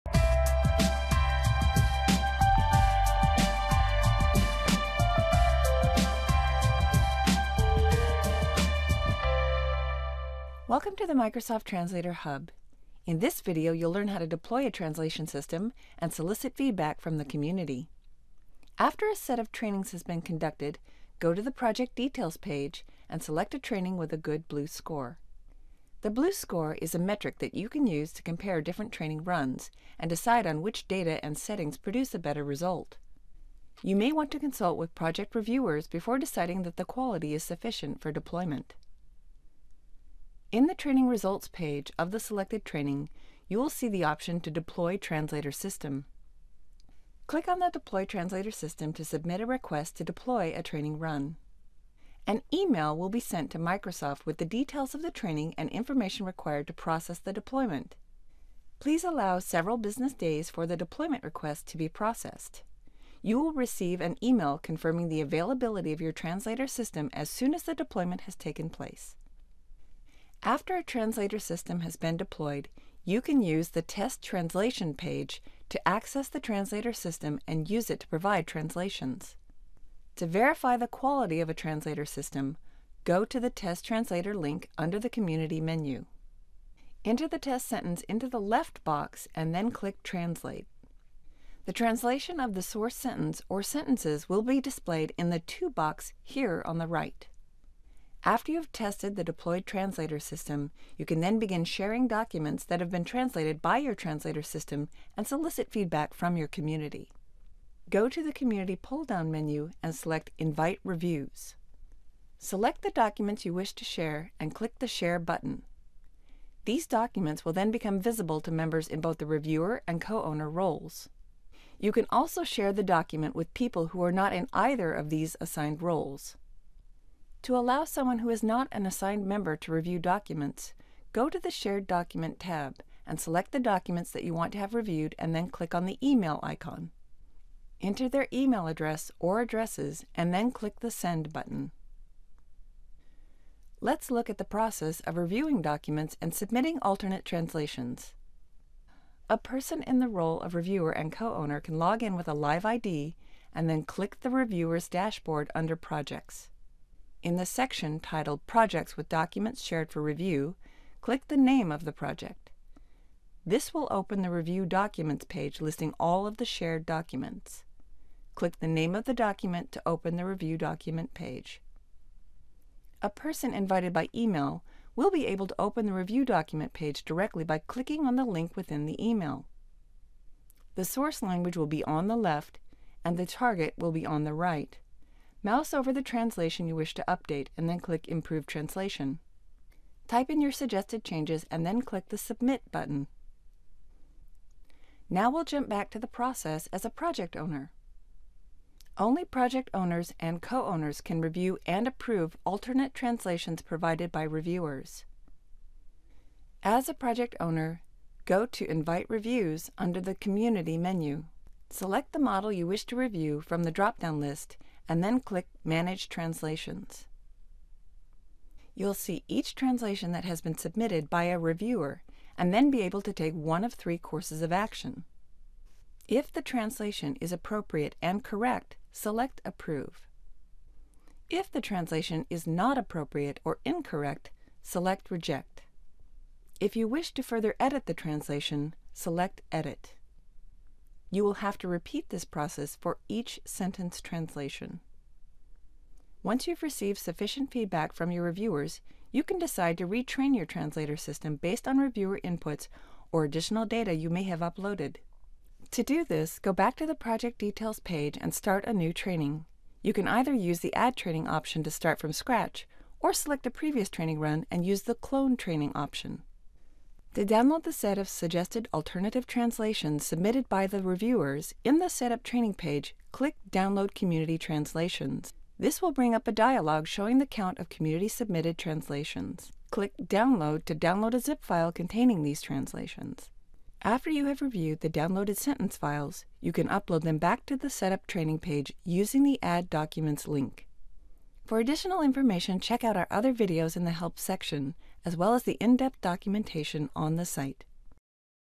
Microsoft Translator Hub - Step Three - Deploying a Translation System
Learn how to deploy a translation system with Microsoft's Translator Hub and invite feedback from your community. Translator Hub was developed for organizations with specific language needs such as language preservation or a specific domain and is designed to put the the power of translation in the hands of users.